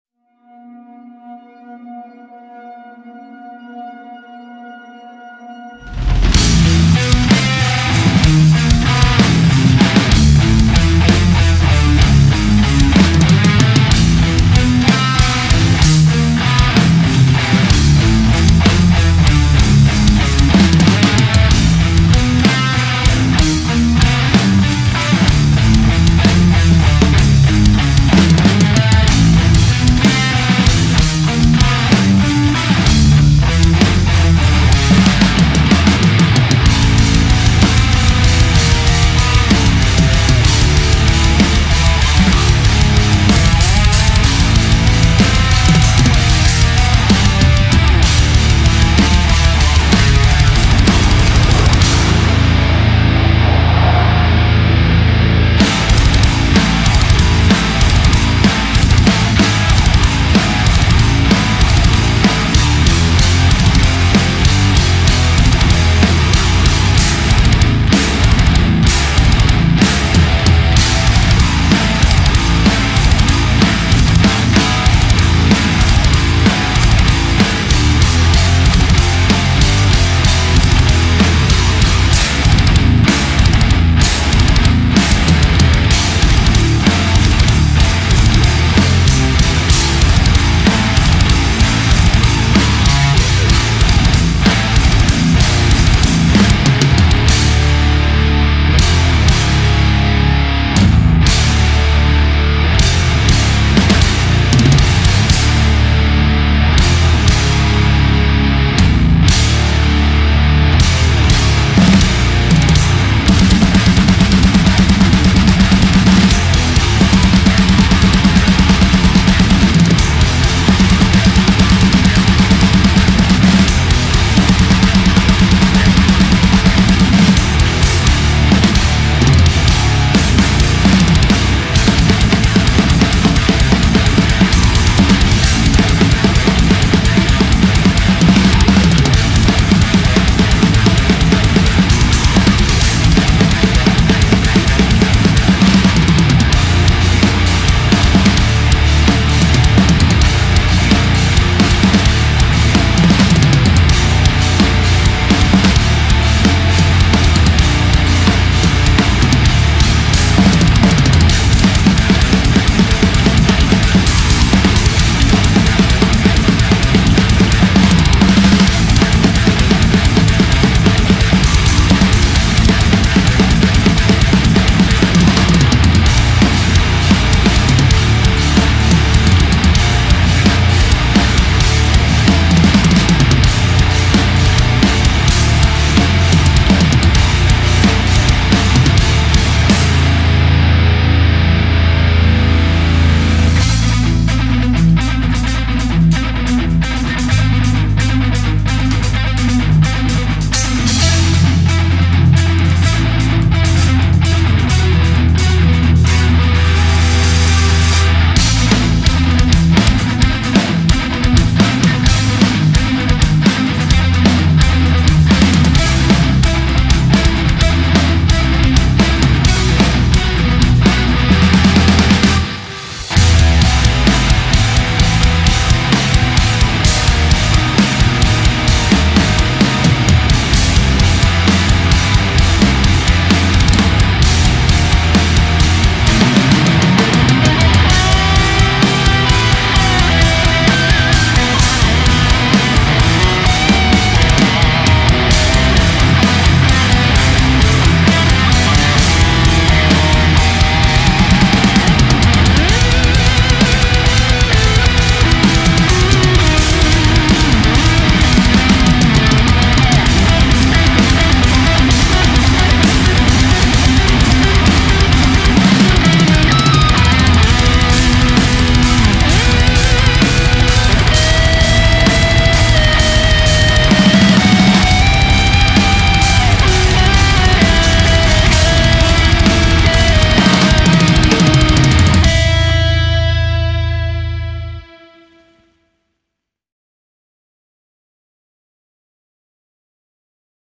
RECORDING METAL DRUMS \M/
A few weeks back I received some guitar tracks from a friend, Loris Caseau from the band Ardenne Heavy and he asked me if I could record some drums for him. I ended up having a real blast doing it and I couldn't be more pleased with the result!

Loris Caseau: Guitars & Composition
Florian Ansion: Bass
Jonas Sanders: Drums

Mix & mastered at Blackout Studio by Jonas Sanders